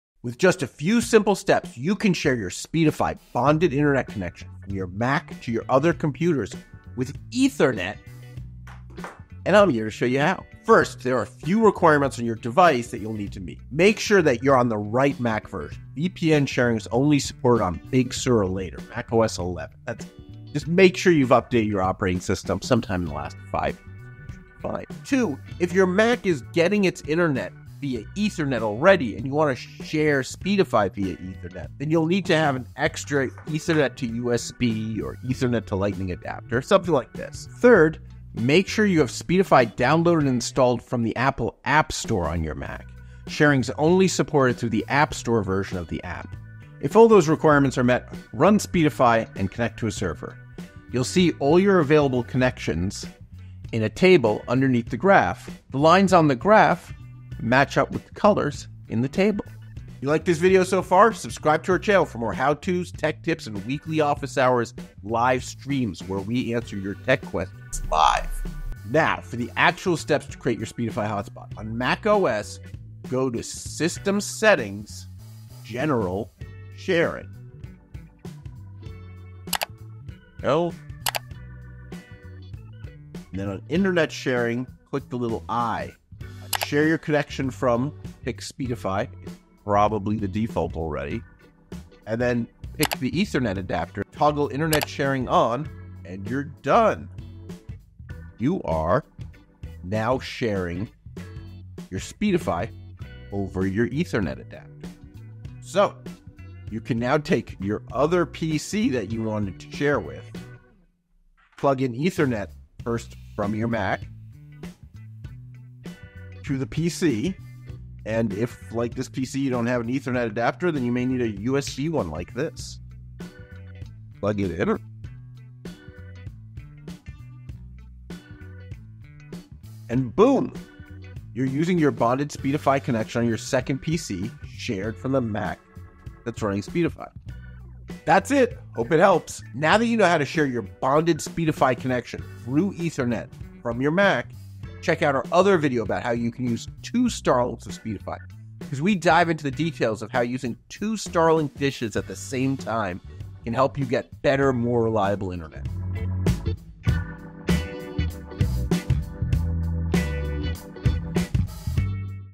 How to Turn your Mac into an Ethernet Router
Speedify not only lets you combine your available Internet connections on your device locally for better speeds and stability, but it also allows you to turn your Windows, MacOS or Linux device into a Speedify Router and share your bonded connection with your phones, TVs or other computers so you could enjoy better connectivity in your entire household or workplace.

With just a few steps you can share your Bonded Speedify Connection from your Mac through Ethernet with your other computers, or run into into a router that can share it even further with more devices!